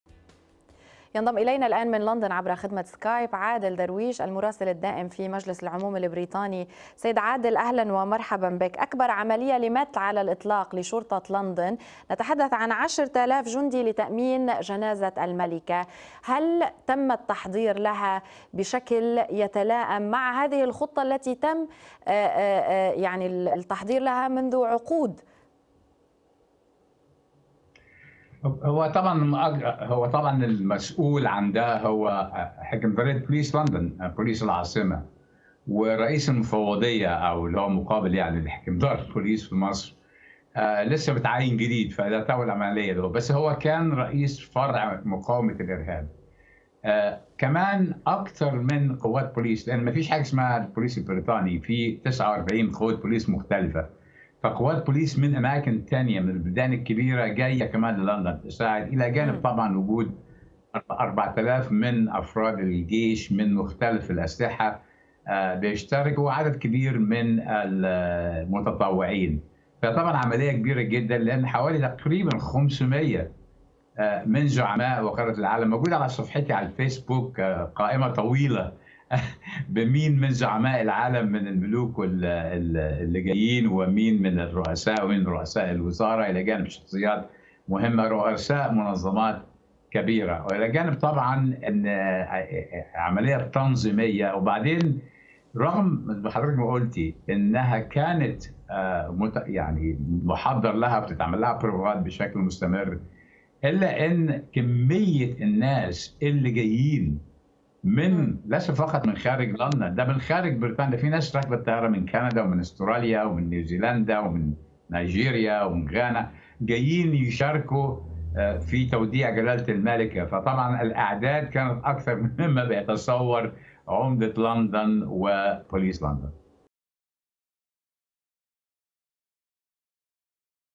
شرطة لندن تتأهب لأكبر تحد أمني منذ "الحرب العالمية"
تستعد قوات_الأمن في العاصمة البريطانية لندن لأكبر مهامها على الإطلاق، لـ تأمين_العاصمة، قبل جنازة الملكة إليزابيث الثانية، وفقاً لضابط كبير.
وقال ستيوارت كوندي، النائب المساعد في شرطة لندن، إن حماية القادة الأجانب وغيرهم من الشخصيات البارزة الذين يحضرون الجنازة سيكون بمثابة «أكبر_عملية_حماية _عالمية» تقوم بها شرطة العاصمة على الإطلاق.
تفاصيل أكثر عن الموضوع مع ضيف قناة الغد من لندن، المراسل الدائم في مجلس العموم البريطاني، عادل درويش.